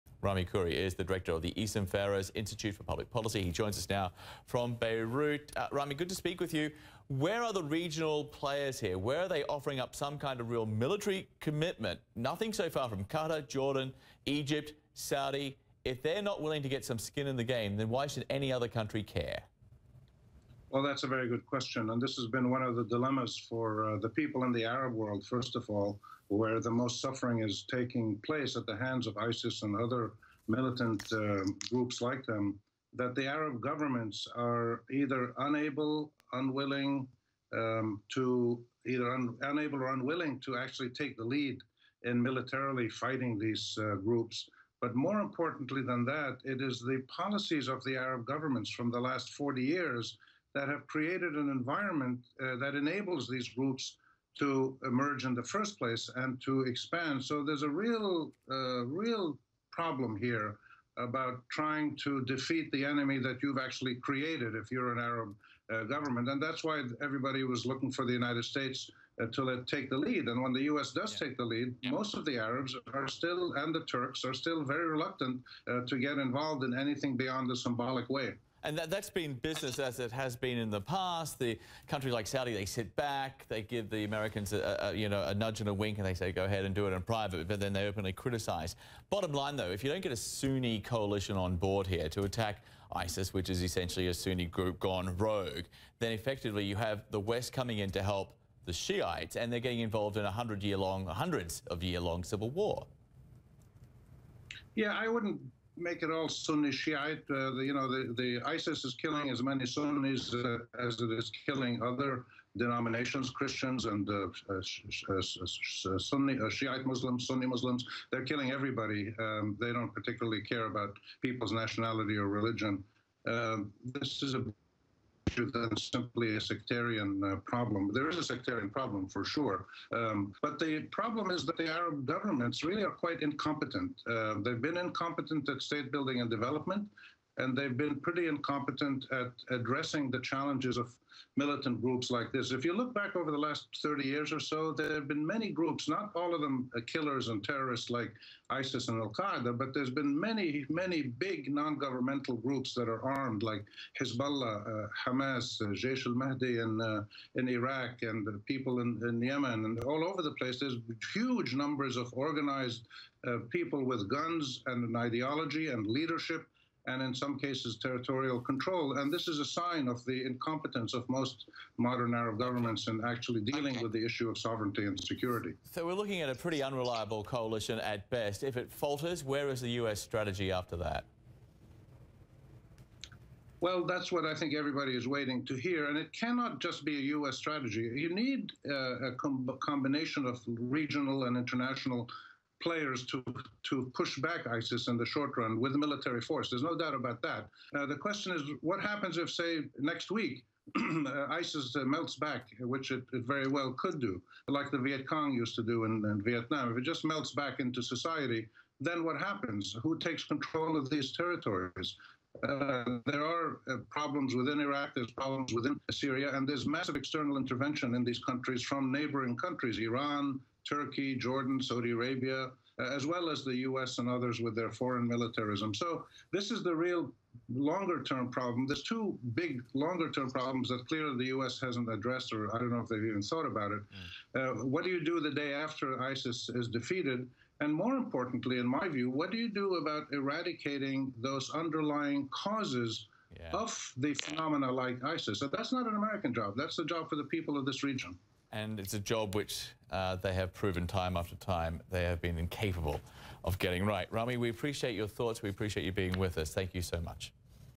Intl Coalition to fight ISIS
CNN talks to Middle East expert Rami Khouri about the international coalition to fight ISIS.